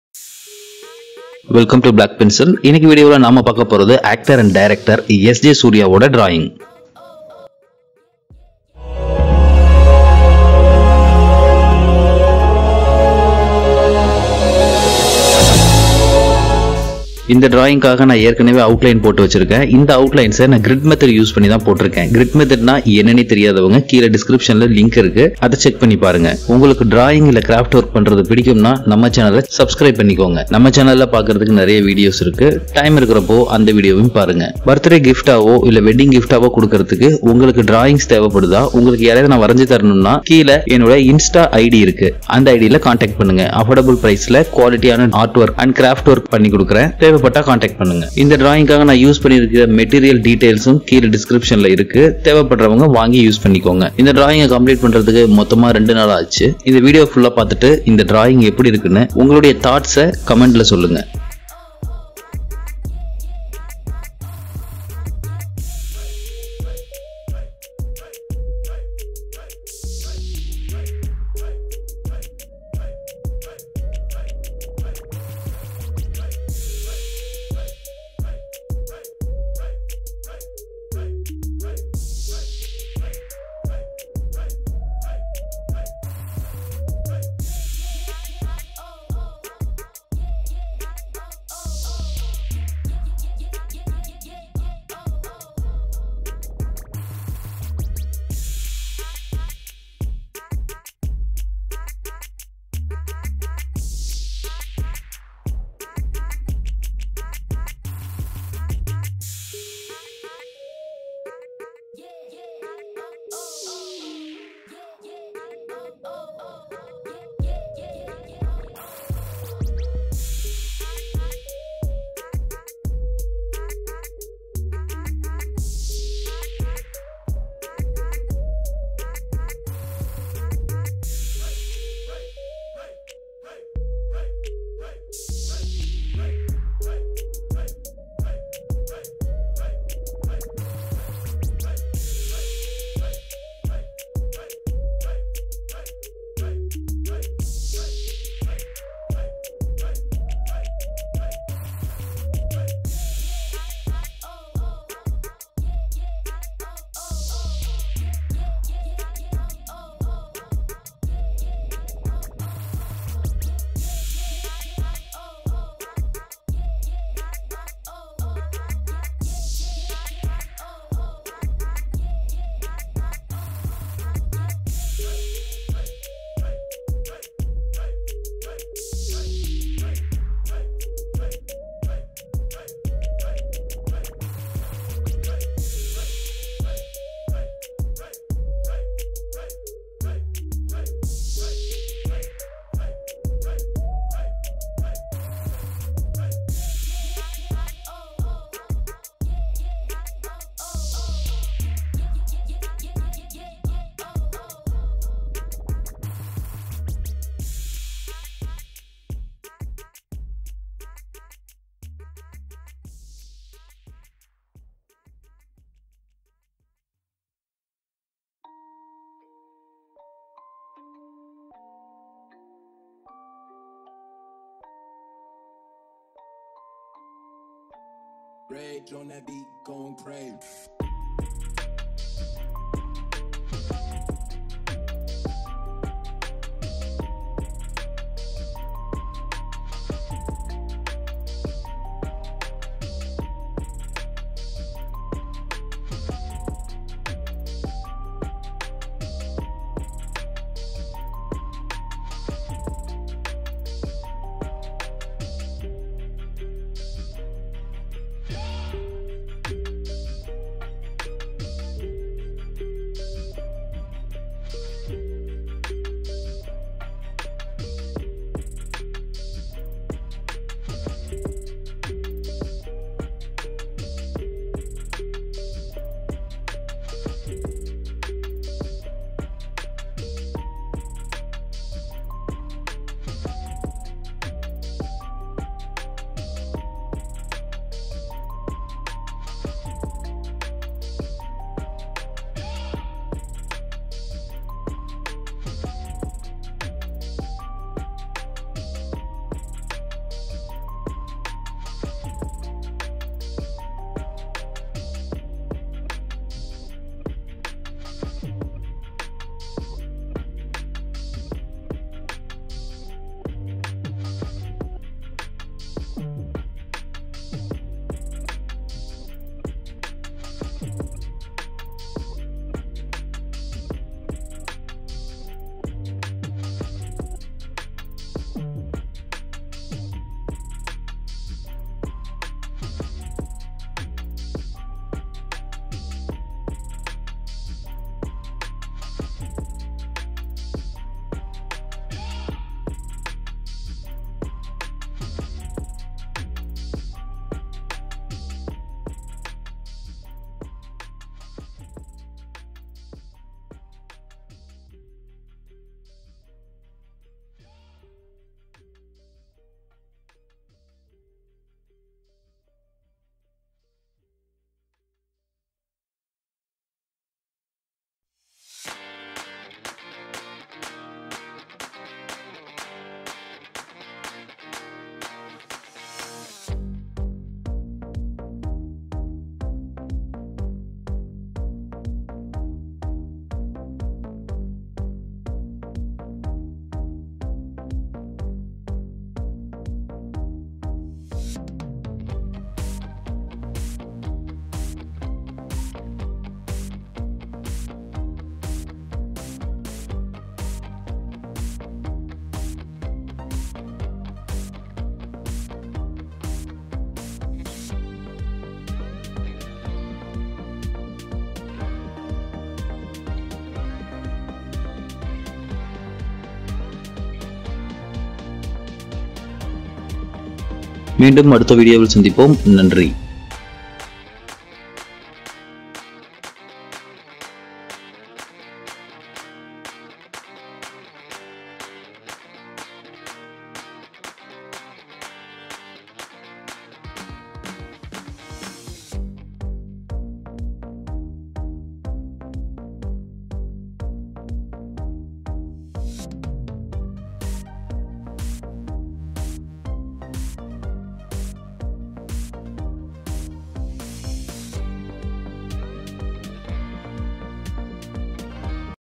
actor SJ Surya pencil drawing | NDrawings
Maanadu Actor & Director SJ Suryah Pencil Drawing easy step by step tutorial | Black Pencil

Materials used:
Steadler Pencils
Paper Stumps
Drawing Paper
Art Eraser
Artline Pencils

Also check
Steadler colour Pencils
Faber castel Pens
Detailing Knife
Water colour Brush